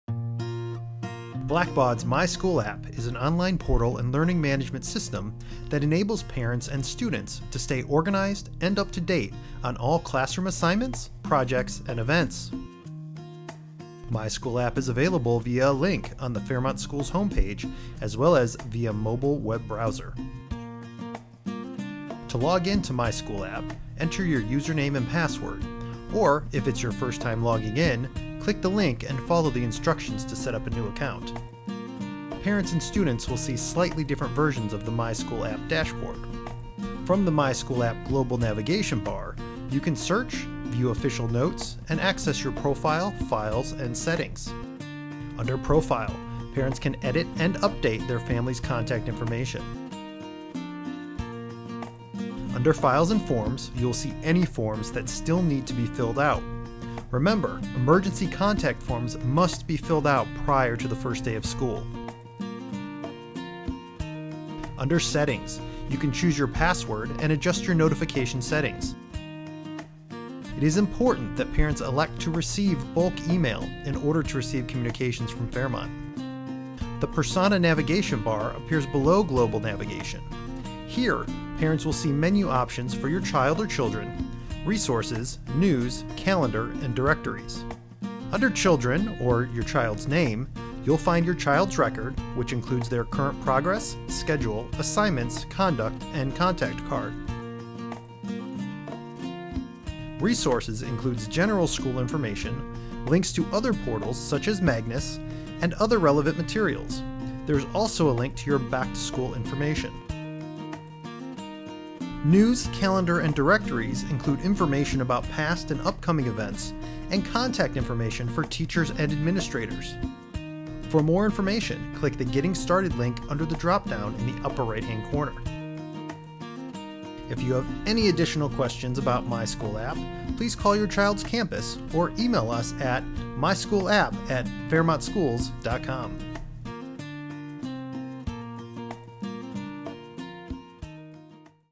Intro to My School App (Fairmont Private Schools)
My School App is an online and mobile portal from Blackbaud that enables parents and teachers at Fairmont Private Schools and Fairmont Preparatory Academy to access and update student information from anywhere.